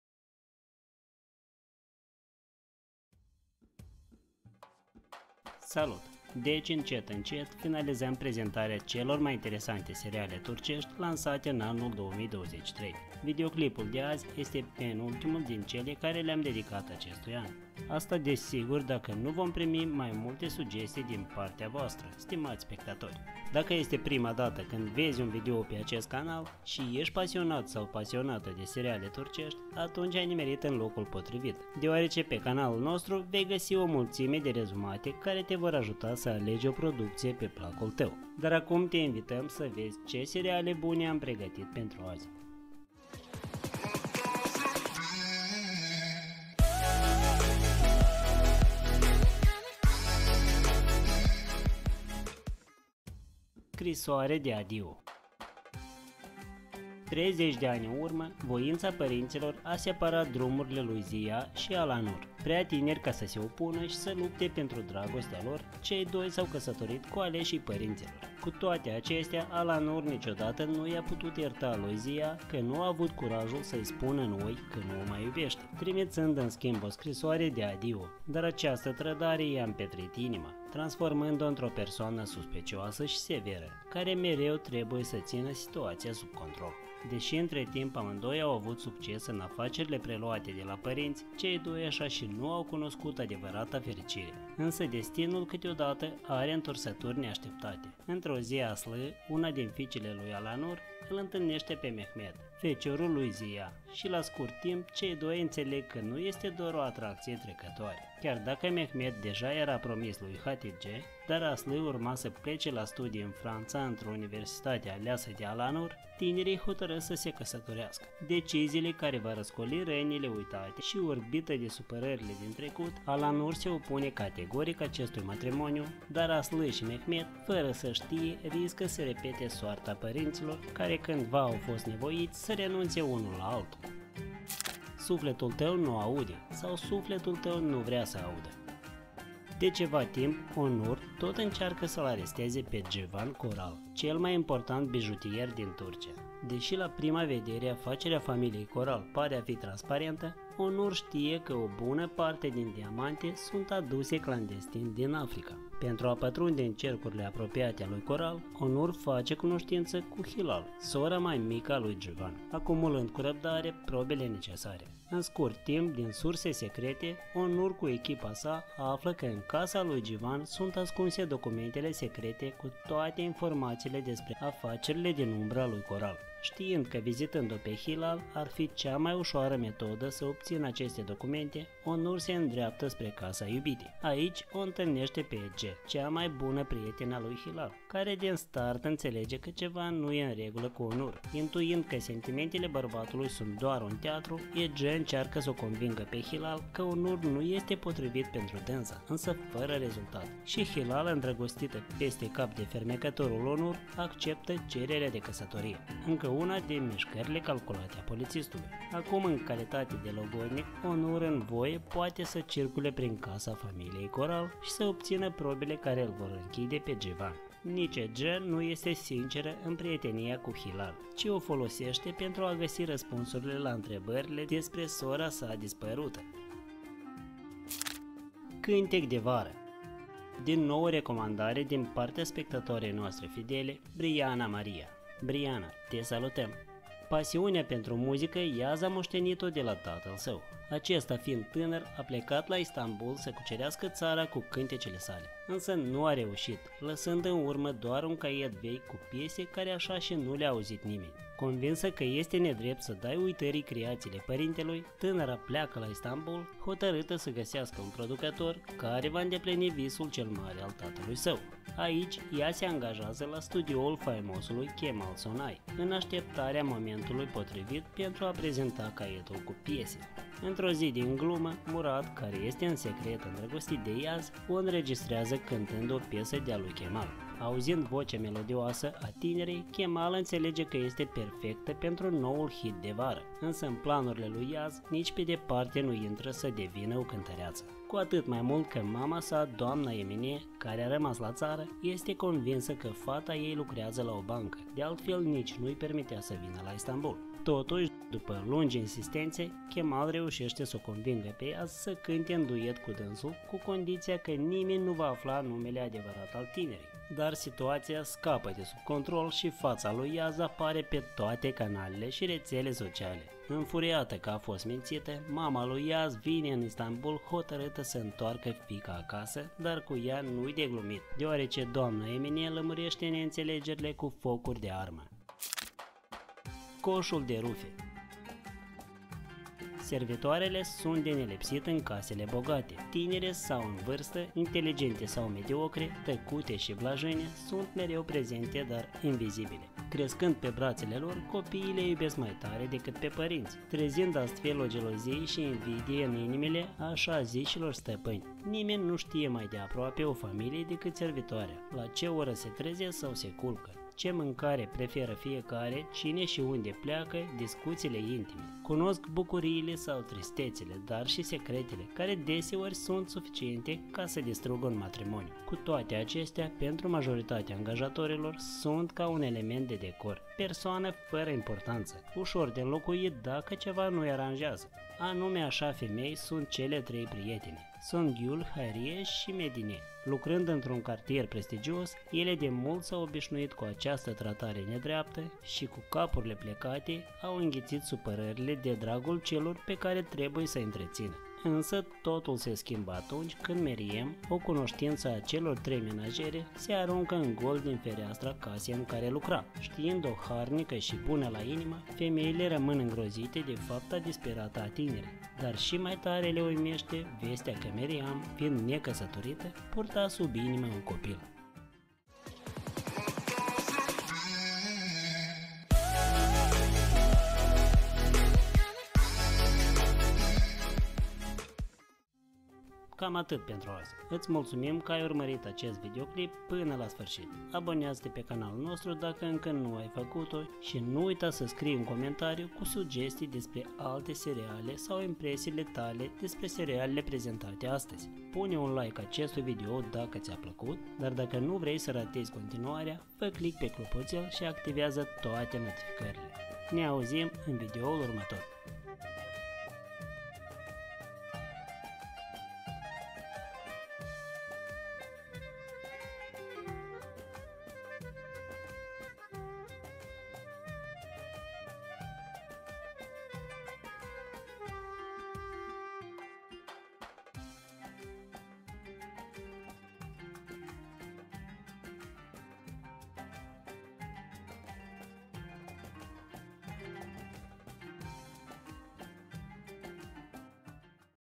Descoperă SERIALE TURCEȘTI care au cucerit inimile în 2023 vol 4 din 5
Salut și bine ai venit pe canalul nostru dedicat filmelor și serialelor.  Deci, încet-încet, finalizăm prezentarea celor mai interesante seriale turcești lansate în anul 2023. Videoclipul de azi este penultimul din cele care le-am dedicat acestui an.
Scrie în comentarii dacă ai văzut careva din filmele de mai sus și dacă da, ce impresie ți-au lăsat 👇